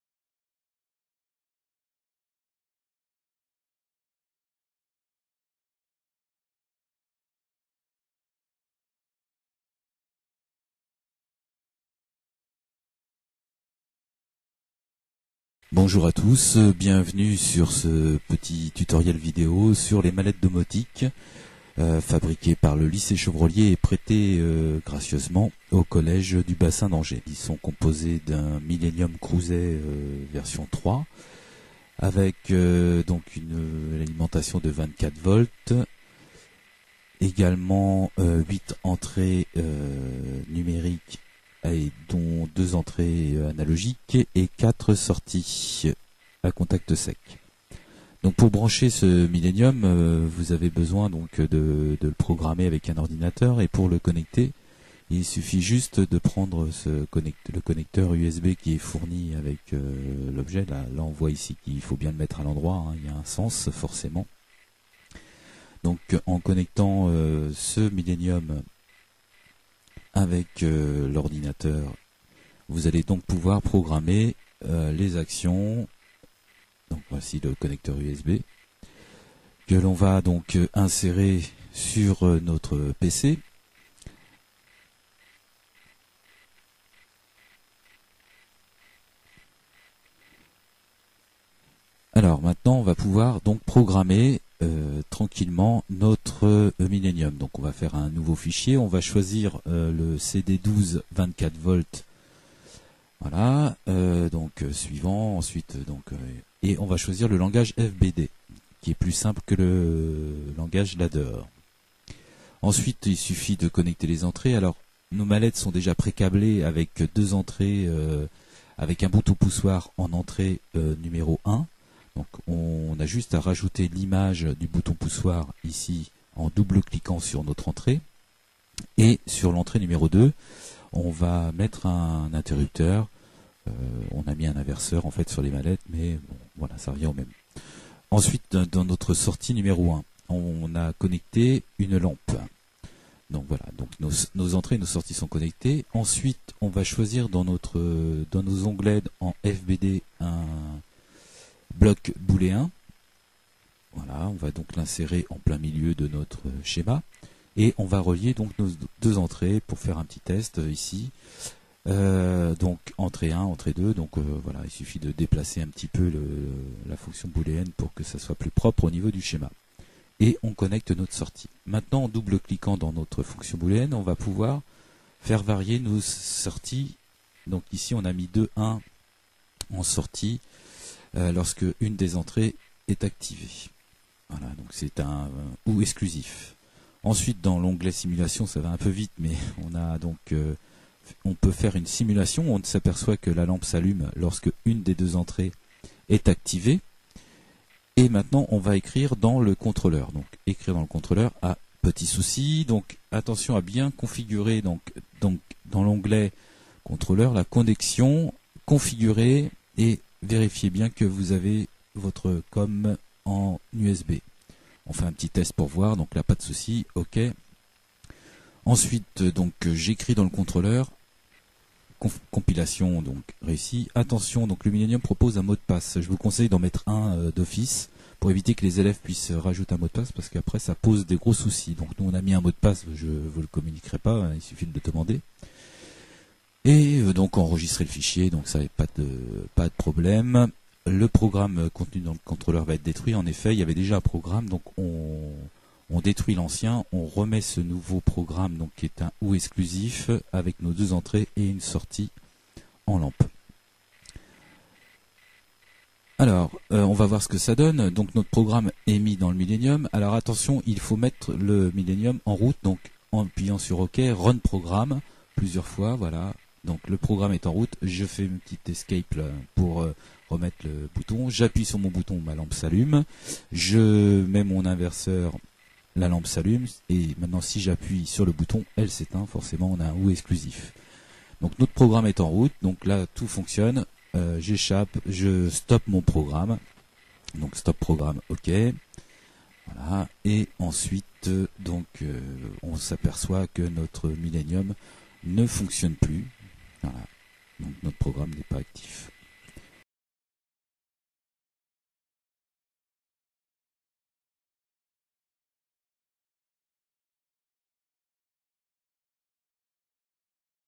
presentation 01
présentation des mallettes domotiques du lycée chevrollier constituées d'un crouzet millenium.